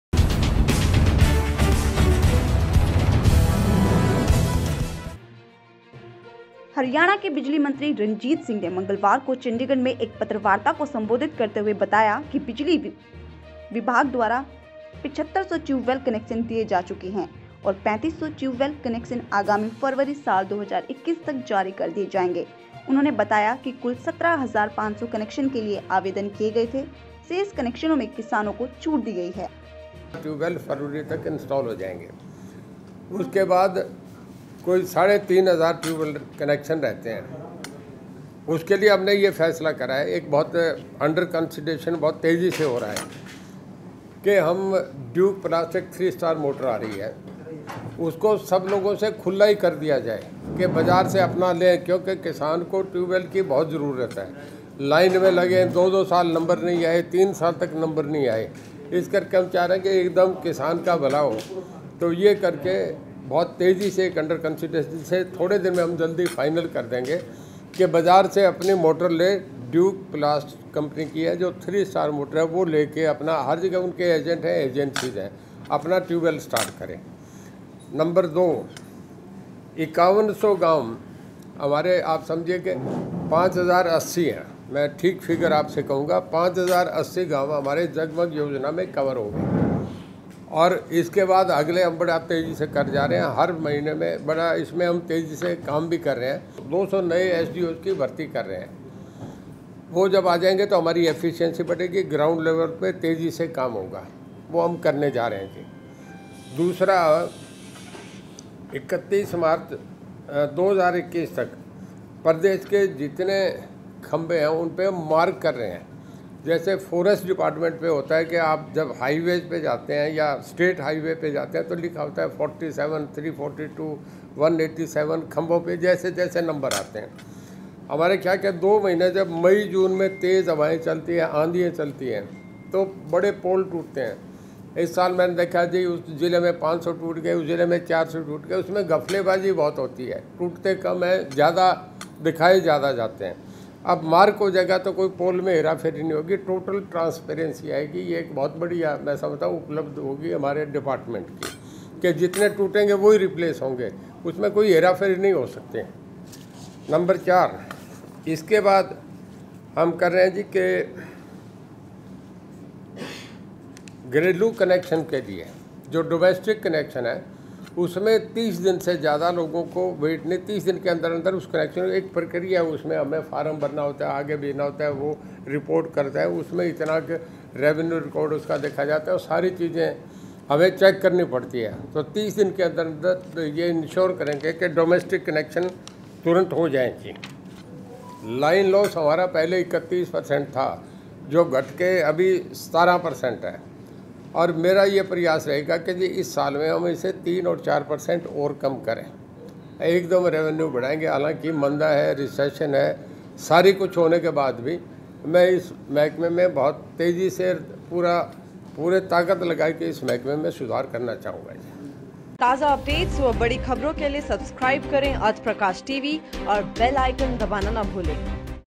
नए साल में बिजली क्षेत्र में होने वाले सुधारों पर मंत्री रणजीत चौटाला ने बताई पूरी योजना
हरियाणा के बिजली मंत्री रणजीत सिंह  ने मंगलवार को चंडीगढ़ में एक पत्रकारवार्ता को सम्बोधित करते हुए बताया कि बिजली विभाग द्वारा 7500 टयूबवैल कनैक्शन दिए जा चुके हैं और 3500 टयूबवैल कनैक्शन आगामी फरवरी, 2021 तक जारी कर दिए जाएंगे । उन्होंने बताया कि कुल 17500 कनैक्शन के लिए आवेदन किये गये थे । शेष कनैक्शनों में किसानों को छूट दी गई है। उन्होंने बताया कि अब किसानों को 3 स्टार मोटर बाजार से खरीदने की छूट दी गई है और ये कनैक्शन भी जल्द ही लगा दिए जायेंगे। बिजली मंत्री ने बताया कि आगामी 31 मार्च, 2021 तक प्रदेश भर के सभी खराब खंभों को ठीक किया जायेगा या बदल दिया जाएगा। इसके अलावा, बिजली विभाग के सभी खंम्भों की निशानदेही (मार्किंग) की जायेगी ताकि मुख्यालय स्तर से इनकी निगरानी की जा सके। उन्होंने बताया कि अब ऐसे सभी खंभों का रिकार्ड भी रखा जाएगा ताकि किसी भी प्रकार का दुरूपयोग न हो सके। इस संबंध में पिछले दिनों बिजली निगमों के वरिष्ठ अधिकारियों की बैठक की गई जिसमें  निर्देश दिए गए कि आगामी 31 मार्च, 2021 तक सभी खराब खंभों को ठीक किया जाए या उन्हें बदला जाए।

उन्होंने बताया कि अब प्रदेश के 5080 गांवों को ‘‘म्हारा गांव-जगमग गांव’’ योजना के तहत 24 घंटे बिजली की आपूर्ति की जा रही है। इस योजना के तहत लगभग 72 प्रतिशत गांवों को कवर कर लिया गया है और इसके अंतर्गत पंचकूला, अंबाला, कुरूक्षेत्र, यमुनानगर, करनाल, गुरूग्राम, फरीदाबाद, सिरसा, रेवाडी व फतेहाबाद जिलों में 24 घंटे बिजली की आपूर्ति की जा रही है। उन्होंने बताया कि शीघ्र ही शेष गांवों को भी इस योजना के तहत 24 घंटे बिजली की आपूर्ति की जाएगी। बिजली मंत्री ने बताया कि बिजली के लाईन लॉस को 30 प्रतिशत से घटाकर 17 प्रतिशत तक लाया गया है और इसे और कम करने की दिशा में सकारात्मक कदम उठाये गये हैं।